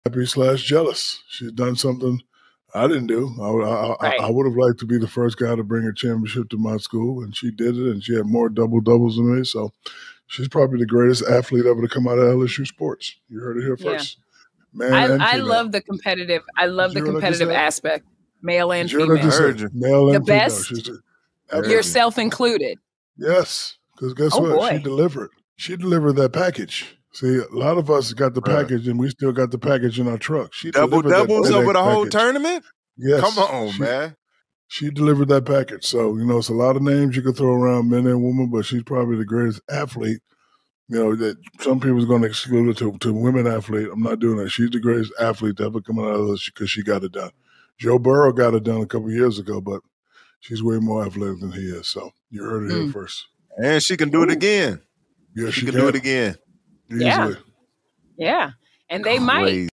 Is Angel Reese The Greatest LSU Athlete Of All Time
Shaq recently made headlines when he named Angel Reese as the greatest athlete of all time at LSU. This past Women’s March Madness Basketball Tournament, she led them to victory in the championship game against Caitlyn Clark and the Iowa Hawkeyes. Not only did they win, but Reese was also named the Most Outstanding Player of the tournament.Shaquille O'Neal's incredible basketball career at Louisiana State University and his thoughts on who he believes is the greatest athlete of all time. As a former LSU basketball star himself, Shaq's opinion on this matter holds immense value and insight.

Shaq had a remarkable career at LSU, where he played from 1989 to 1992. During his time at the university, he led the Tigers to the NCAA tournament twice, was named the SEC player of the year in 1991 and 1992, and was a consensus All-American in 1992. He went on to have a successful NBA career, winning four championships and becoming one of the most dominant players in basketball history.

With his vast experience and knowledge of the sport, Shaq is qualified to speak on who he believes is the greatest athlete of all time. In this video, he shares his thoughts on the matter, providing valuable insights and analysis.

Click like.
Click share.
Argue in the comments.